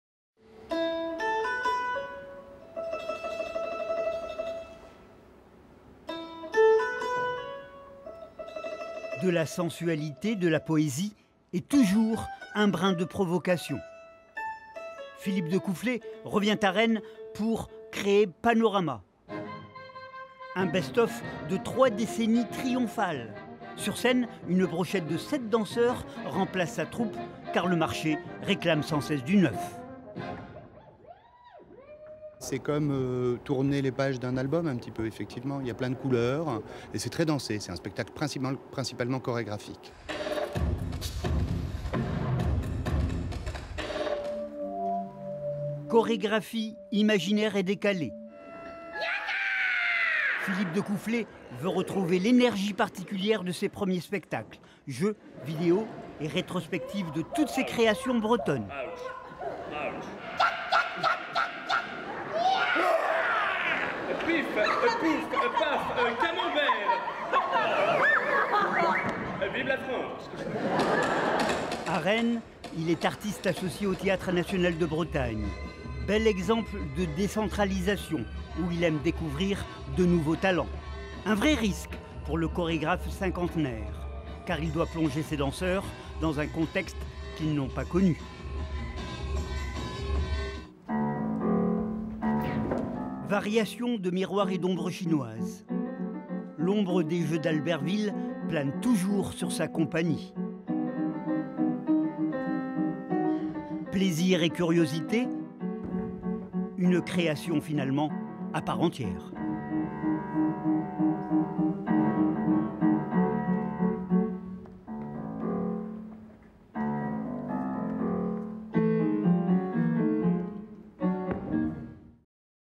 Panorama, un best-of de Decouflé à Rennes (35)
Philippe Découflé présente un programme dansé vendredi 4 mai, au TNB de Rennes (35). On y retrouve les inspirations fantasques et les expérimentations artistiques du chorégraphe.